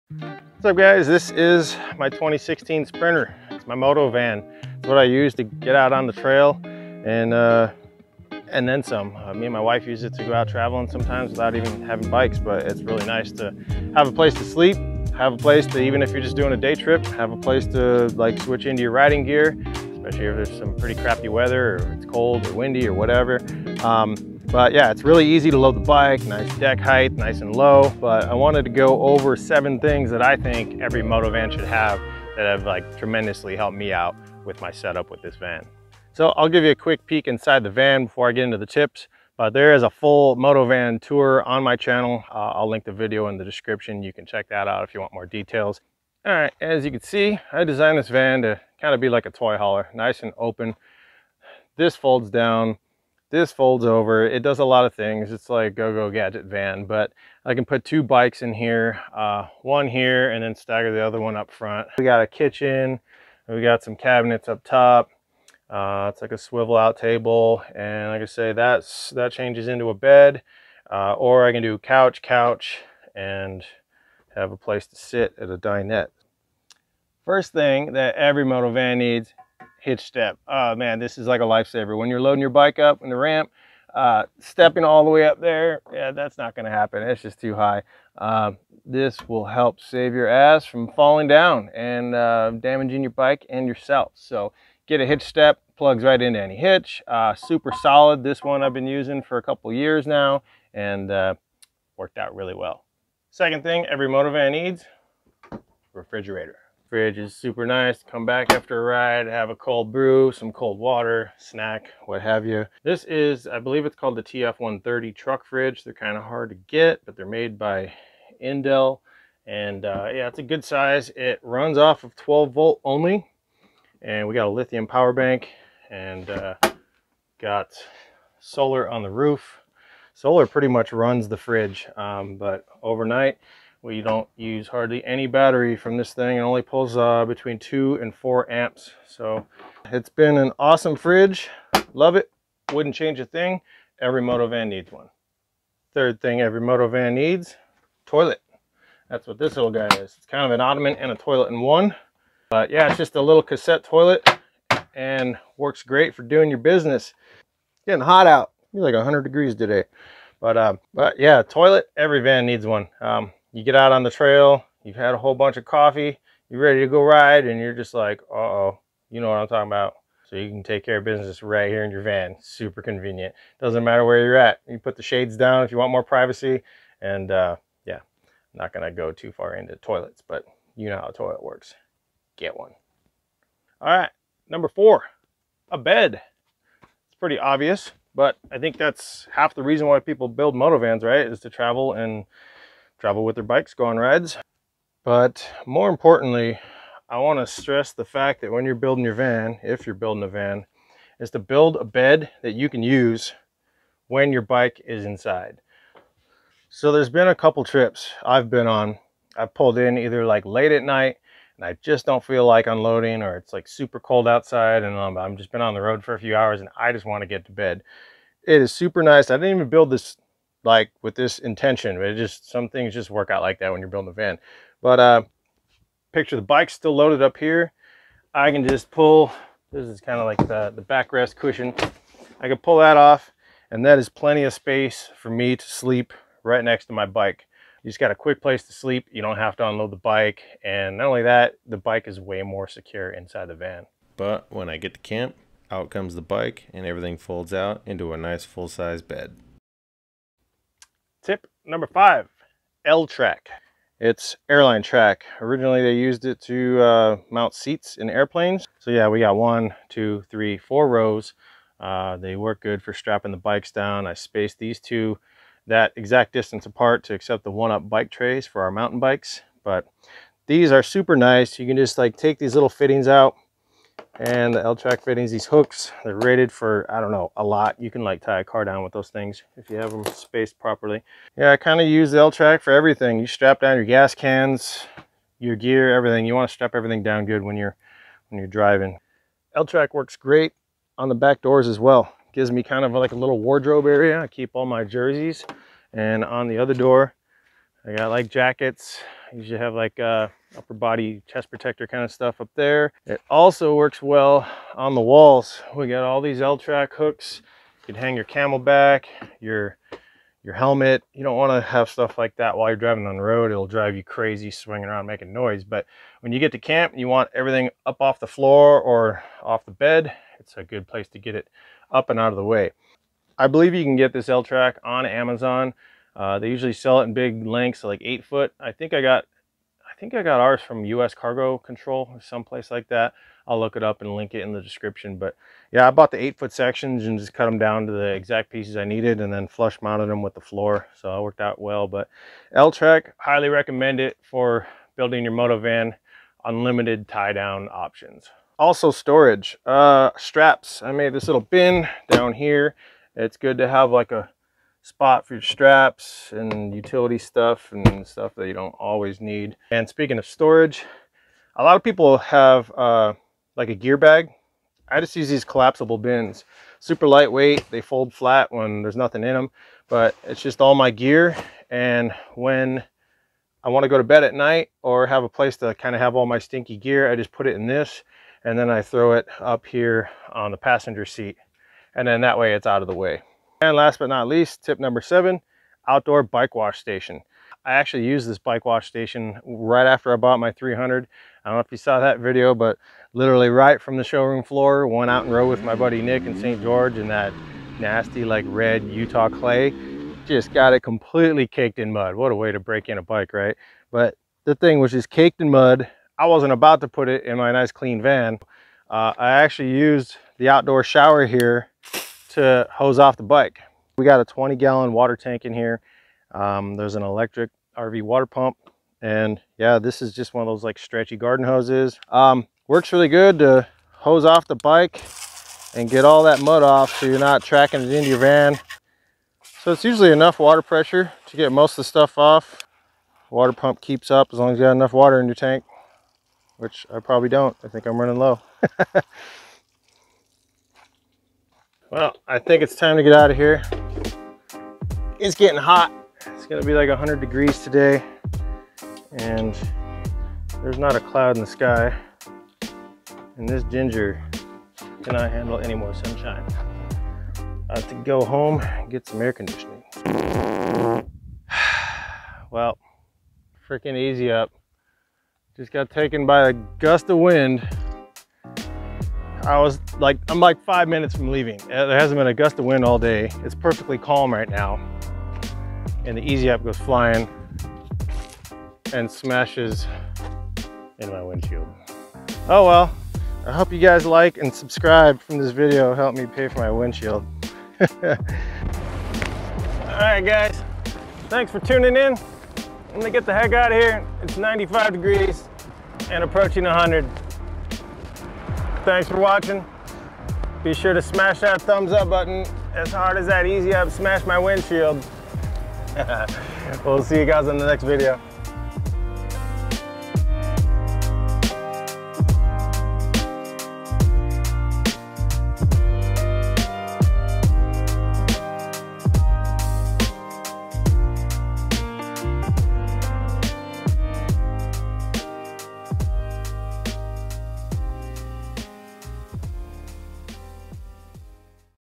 7 Things Every MOTOVAN Needs
Some things are a must-have in my converted moto van. After building out our Sprinter and using it for a few years, I discovered how important some parts of the conversion have been. If you'd like to see more of the van, check out my moto van tour video or see the full, detailed buildout posts on my blog.
Hitch Step
Camco cassette toilet
Recessed L Track: 8 ft
Angled L Track
This battery rocks! 300aH LiFeP04 battery